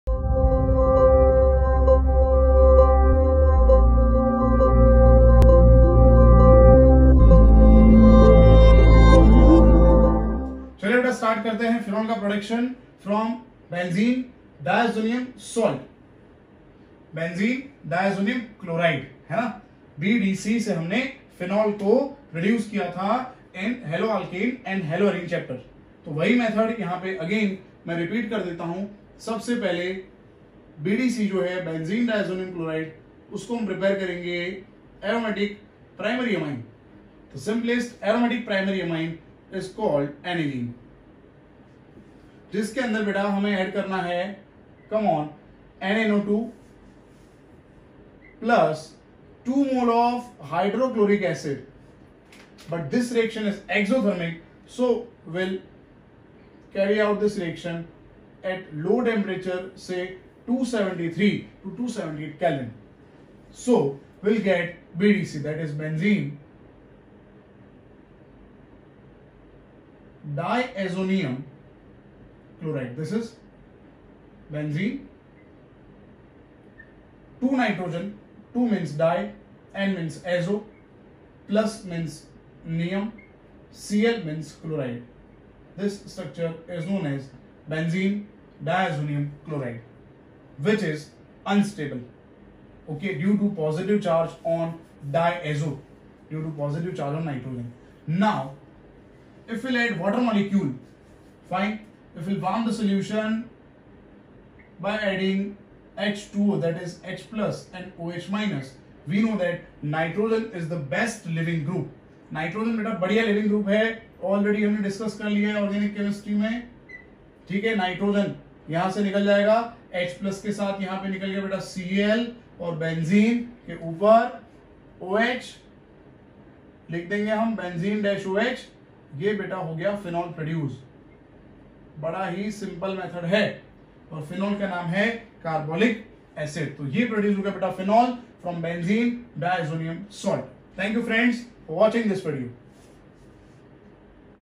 Alcohol,phenol and ether || L 11|| Phenol from BDC || NCERT||NEET|| JEE
Chapter 11
Alcohol, phenol and ether
Lecture 11
Preparation of phenol from Benzene diazonium chloride is explained in the lecture .
Go through the video .
Write all the important topics and try to remember it. NCERT book is our BHAGWAT GITA .
Do follow it .

Camera used for making video lecture : iphone 11max pro

Mike used for making video lecture :
Boya BY-M1